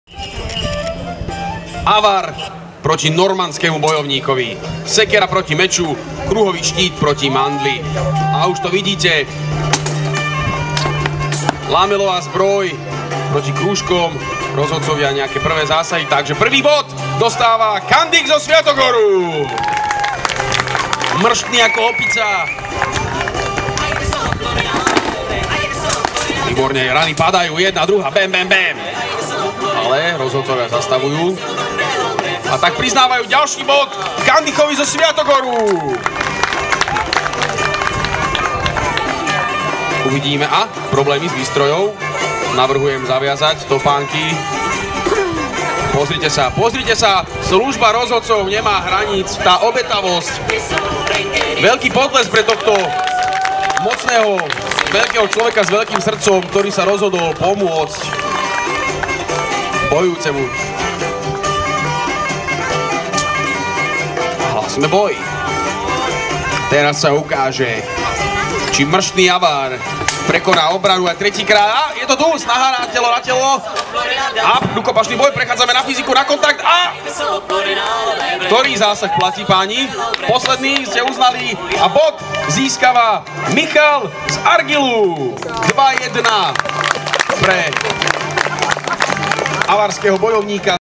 Medieval warrior with round shield and pole axe dueling one with kite shield and one handed sword
Michal from Argyle with kite shield against Kandych with round shield and pole axe. Duel was a part of medieval tournament on Pribinova Nitrawa festival on Nitra castle, Nitra city, Slovakia, july 2017.
Forget Game of thrones, LARPs and COSplay, this is REAL THING ! :)